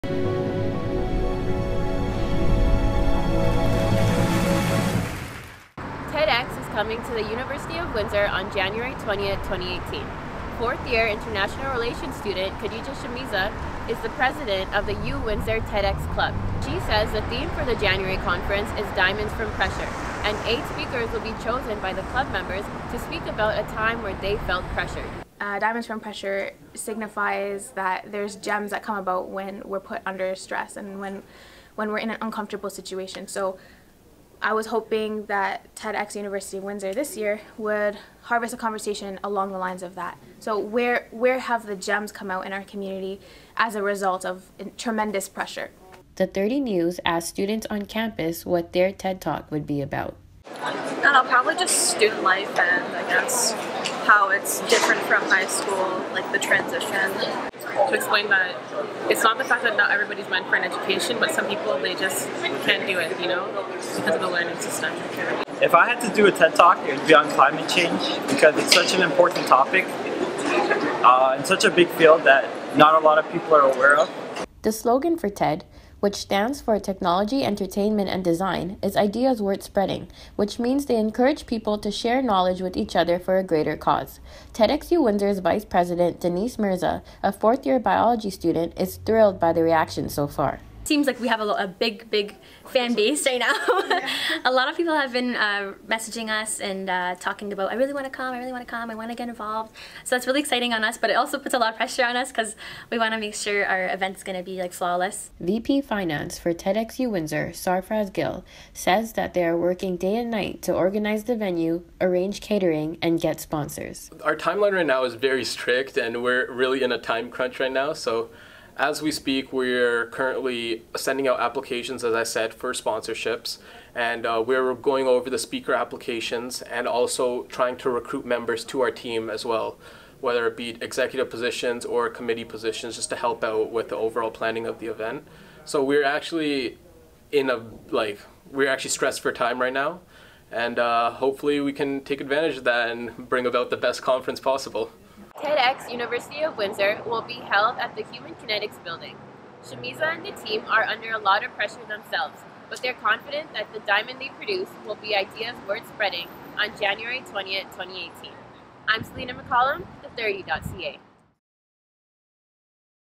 TEDx University of Windsor Is Coming to Campus In January 2018
TEDx, an event under the annual conference TEDtalks, is coming to the University of Windsor on January 20th, 2018 for members of the Windsor community to share their ideas.

Khadija Shamisa, a 4th year student majoring in International Relations, is the president of the club at the university. The theme for the conference is Diamonds from Pressure and eight speakers chosen by the club members will speak about a time they felt pressured.

All 8 speakers will have different backgrounds such as business, engineering, or the arts. The goals is to have each speaker use their background knowledge to speak to a time they overcame a situation.

Reporter/Interviewer: Selina McCallum
Shot and edited by Selina McCallum